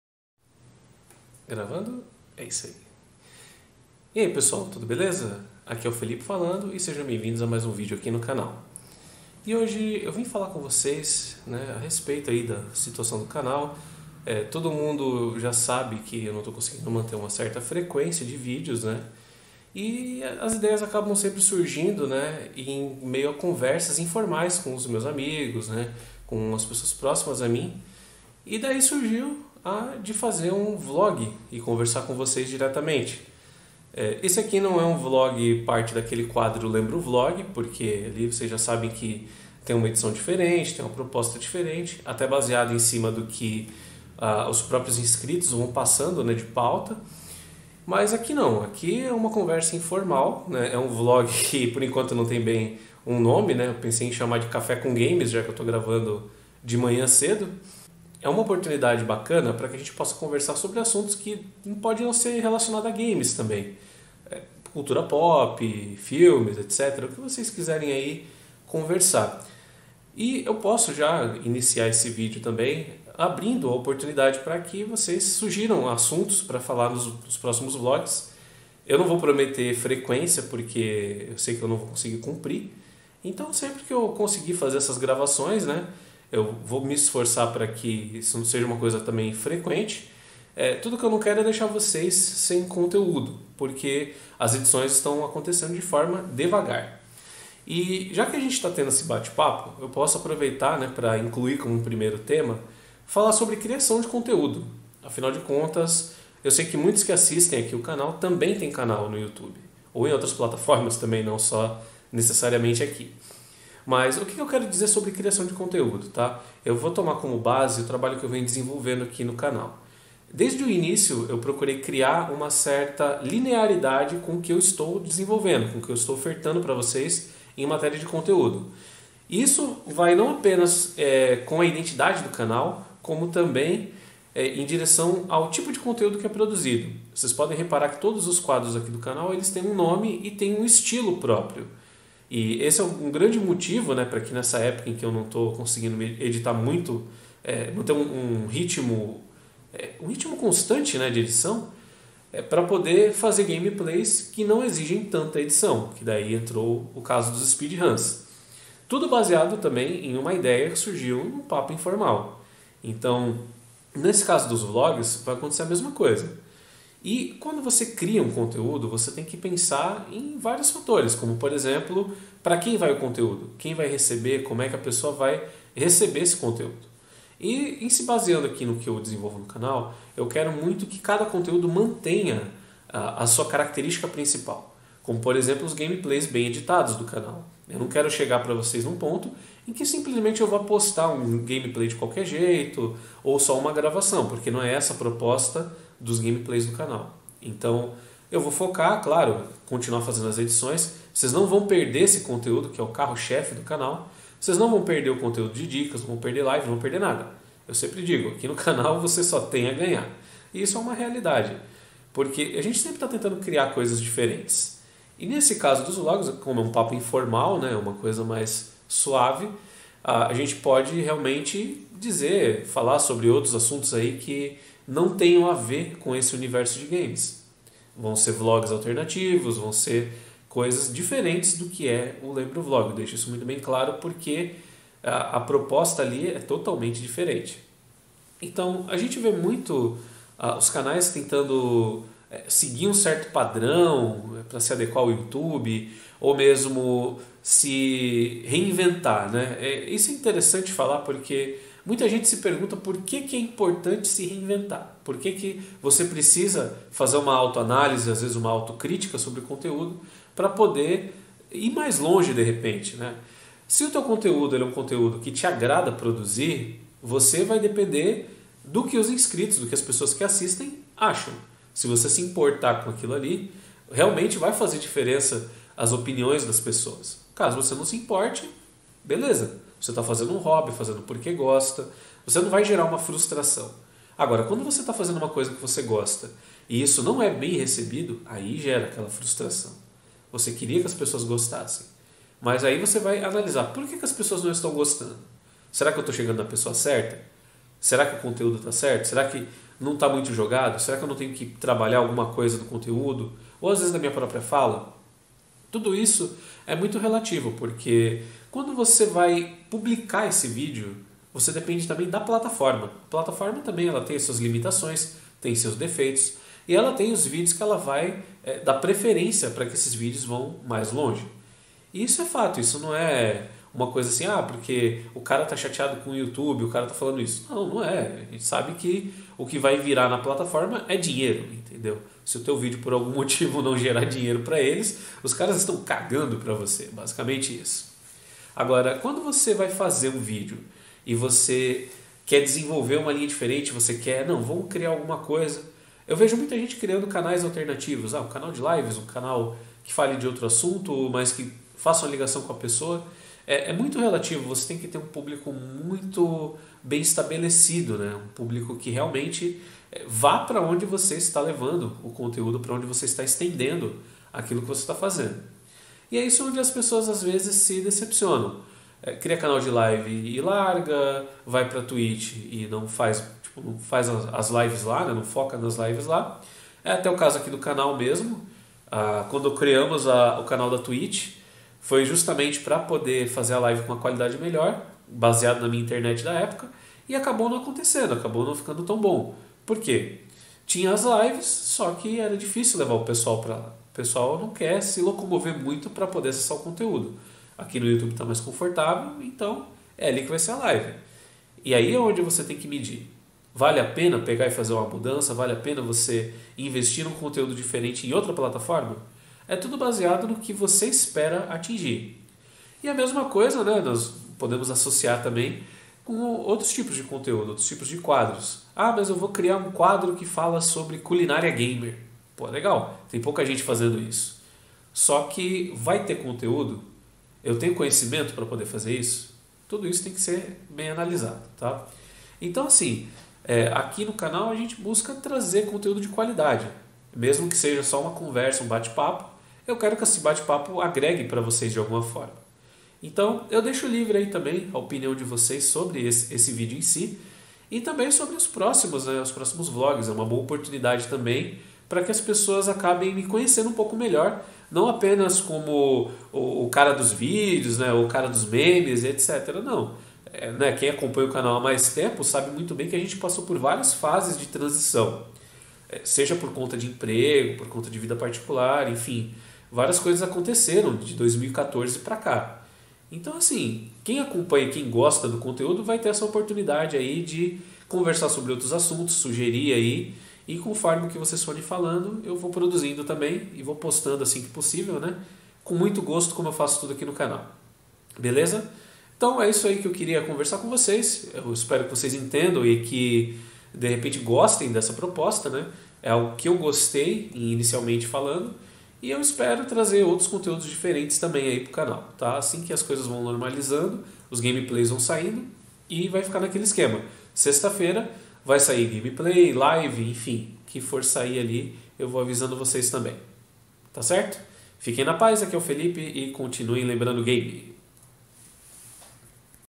☕+🎮 = Canal e Conteúdo
Hoje vamos para um papo relax com um vlog falando um pouco sobre o canal e propondo aos inscritos uma nova ideia de conteúdo.

PRECISA DE EDIÇÃO?

Fique na paz e continue LEMBRANDO O GAME!